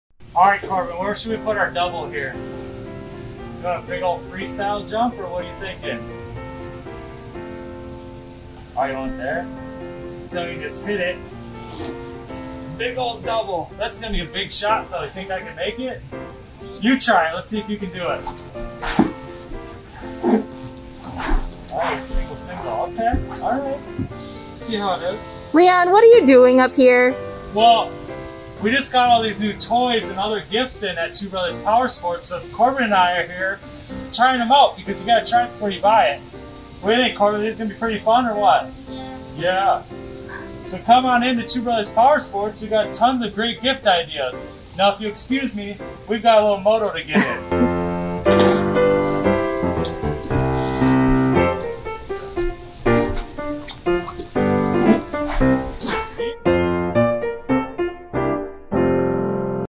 2Brothers Powersports Has Powersport Toys For Everyone!
Start shopping early this year for everyone on your list! 2Brothers Powersports in Onalaska, Wisconsin is the best big kid toy store in the area. We offer Ski-Doo Snowmobiles and Snowblowers for the big kids and toys for the little kids!